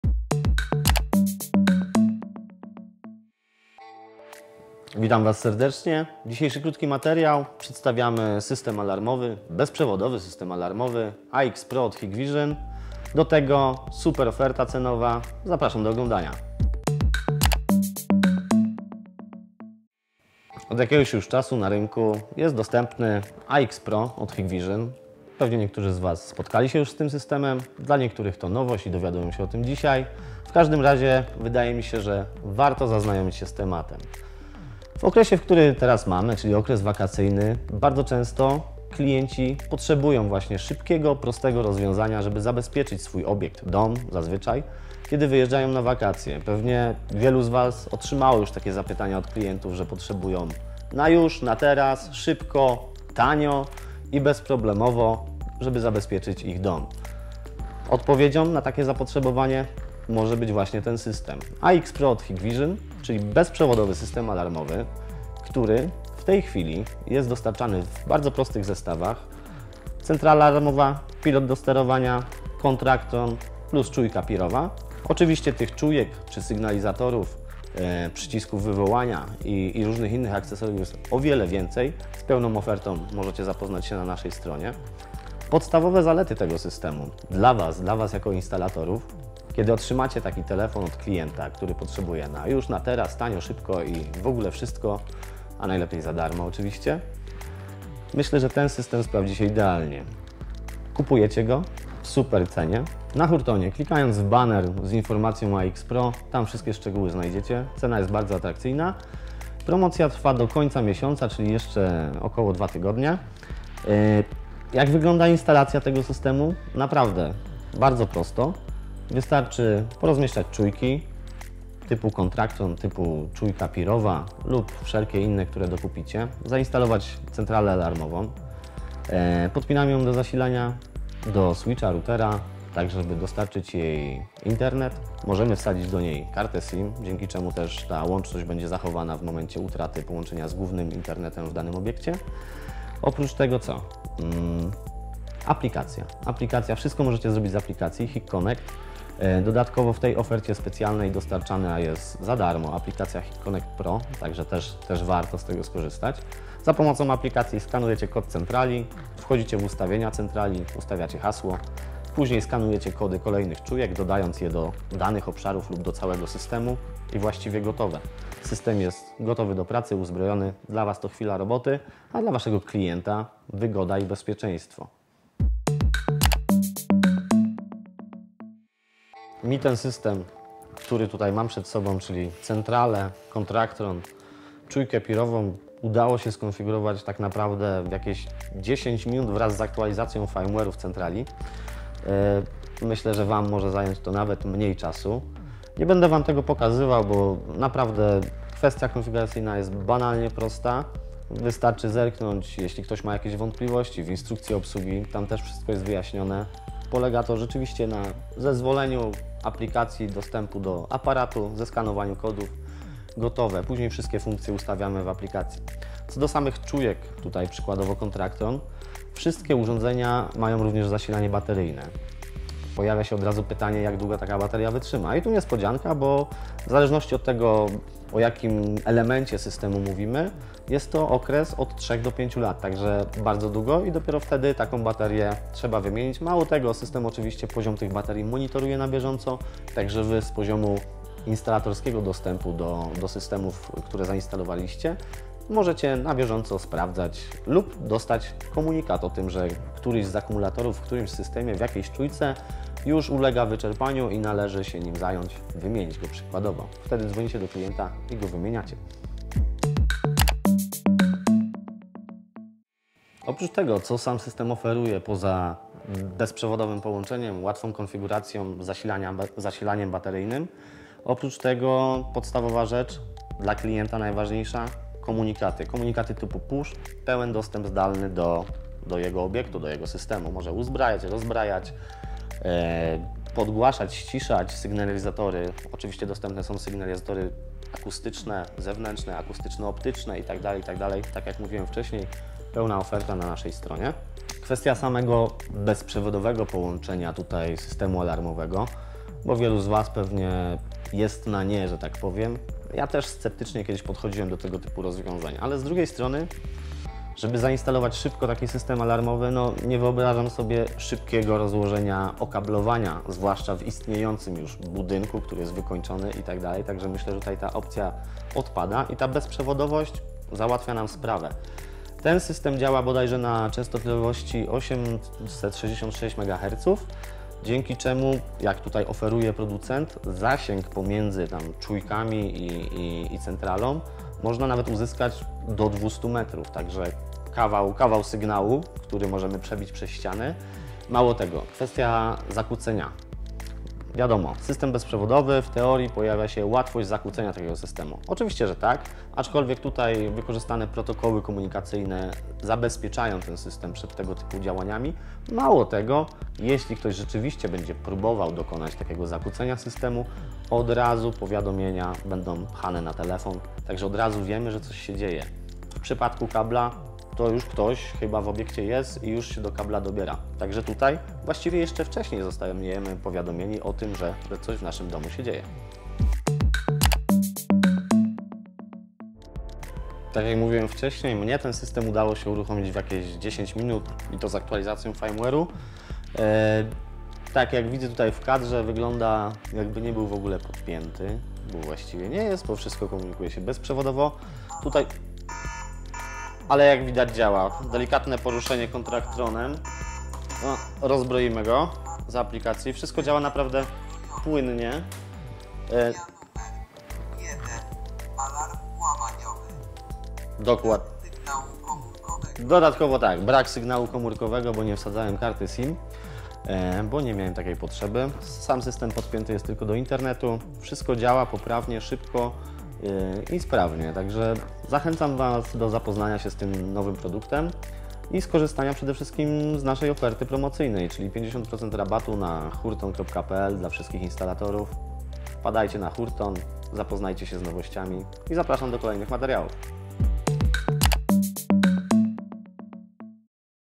Hikvision AX PRO - Bezprzewodowy System Alarmowy DS-PWA64-Kit-WE
🎥W filmie prezentacja bezprzewodowego systemu alarmowego AX PRO od Hikvision.
Model: DS-PWA64-Kit-WE

Niebawem kolejne filmy! 👀
👉 Hurton w internecie: